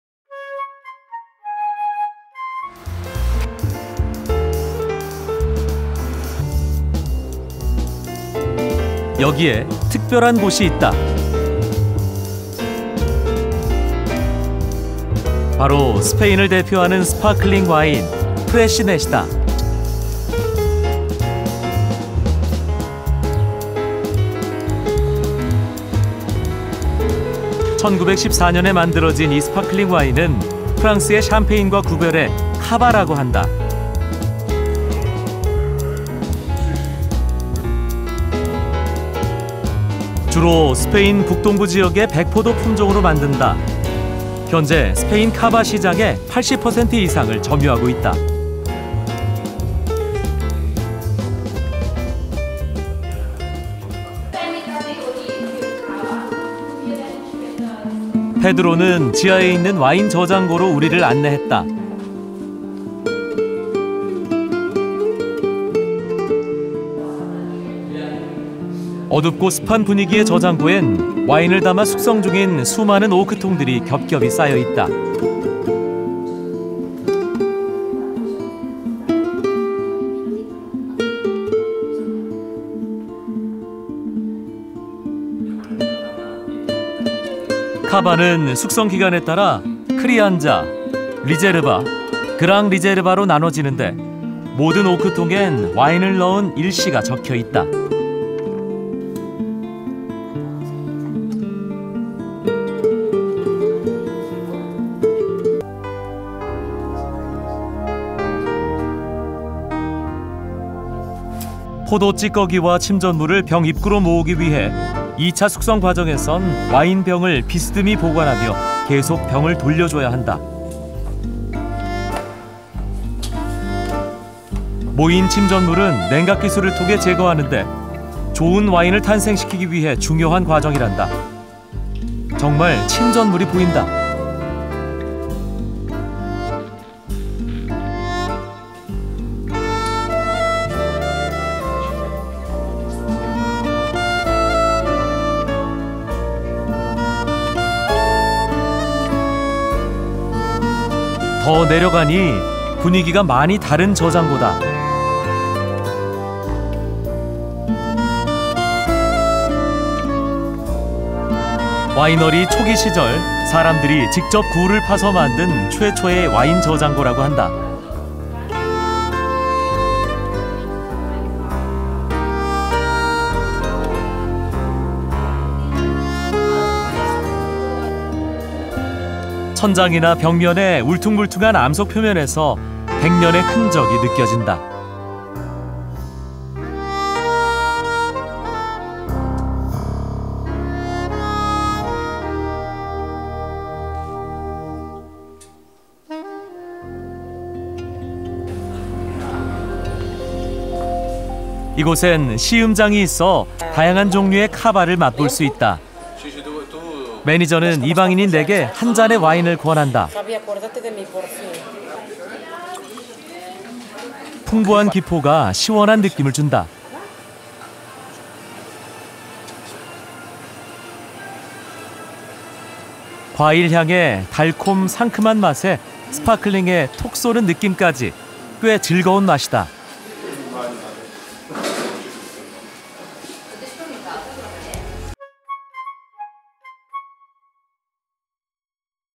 【K】Spain Travel-Barcelona[스페인 여행-바르셀로나]스파클링 와인, 프레시넷/Sparkling Wine/Freixenet/Cava/Winery
[한국어 정보]
이곳에 특별한 곳이 있다. 바로 스페인을 대표하는 스파클링와인, ‘프레시넷’이다. 1914년에 만들어진 이 스파클링 와인은 프랑스의 샴페인과 구별하여 ‘카바’라고 한다. 주로 스페인 북동부 지역의 백포도 품종으로 만들며 현재 스페인 카바 시장의 80% 이상을 점유하고 있다. 페드로는 지하에 있는 와인 저장고로 우리를 안내했다. 왠지 모를 어둡고 습한 분위기의 저장고엔 와인을 담아 숙성 중인 수많은 오크통들이 겹겹이 쌓여있다. 카바는 숙성기간에 따라 크리안자(crianza), 리제르바(reserva), 그랑 리제르바(gran reserva)로 나누어지는데 모든 오크통엔 와인을 넣은 일시가 적혀 있다. 포도 찌꺼기와 침전물을 병 입구로 모으기 위해 2차 숙성 과정에선 와인 병을 비스듬히 보관하며 계속 병을 돌려주어야 한다. 모인 침전물은 냉각기술을 통해 제거하는데 좋은 와인을 위한 중요한 과정이란다. 정말 침전물이 보인다. 더 내려가니 분위기가 많이 다른 저장고다. 와이너리 초기 시절 사람들이 직접 굴을 파서 만든 최초의 와인저장고라고 한다. 천정이나 벽면의 울퉁불퉁한 암석표면에서 100년의 흔적이 느껴진다. 이곳엔 시음장이 있어 다양한 종류의 카바를 맛볼 수 있다. 매니저는 이방인인 한 잔의 와인을 권한다. 풍부한 기포가 시원한 느낌을 준다. 과일향의 달콤 상큼한 맛에 스파클링의 톡 쏘는 느낌까지 꽤 즐거운 맛이다.

There is a special place here. Sparkling right to represent Spanish wines , the ' fresh four . This sparkling wine made ​​in 1914 to distinguish it from the champagne in France is called ' cava ' . Mainly white grape varieties creates a region of northeastern Spain currently occupies more than 80% of Spanish Cava market . Pedro led us to a wine cellar in the basement . Somehow you do not know the dark, humid atmosphere of the cellar yen put a lot of wine barrels are stacked under layers of maturation . Cava aging period , depending on the creative Anja (crianza), rijereuba (reserva), divided into Grand rijereuba (gran reserva) all oak yen is written into a temporary wine. Marc and sediment to collect in the bottle secondary entrance In the aging process and keep a bottle of wine should continue rotating the bottle at an angle .

Hay un lugar especial aquí . Derecho espumoso para representar los vinos españoles , la " fresca cuatro . Este vino espumoso hecho en 1914 para distinguirla de la champaña en Francia se llama ' cava ' . Principalmente variedades de uva blanca crea una región del noreste de España ocupa actualmente más del 80 % del mercado español Cava . Pedro nos llevó a una bodega en el sótano. De alguna manera usted no sabe la oscuridad , atmósfera húmeda del yen bodega de poner un montón de barriles de vino se apilan bajo capas de maduración. Cava de envejecimiento periodo , dependiendo del Anja creativa ( crianza ) , rijereuba ( reserva ) , dividido en Gran rijereuba ( gran reserva ) todas yenes roble está escrito en un vino temporal. Marc y los sedimentos se acumulen en la entrada secundaria botella en el proceso de envejecimiento y mantener una botella de vino debe seguir girando la botella en un ángulo.

[Information]
■클립명: 유럽099-스페인13-13 스페인 대표 스파클링 와인, 프레시넷/Barcelona/Sparkling Wine, Freixenet/Cava/Winery/Crianza/Reserva/Gran Reserva
■여행, 촬영, 편집, 원고: 윤영진 PD (travel, filming, editing, writing: KBS TV Producer)

[Keywords]
유럽,Europe,유럽,스페인,Spain,España,에스파냐,윤영진,2015,3월 March,바르셀로나,Barcelona,Barcelona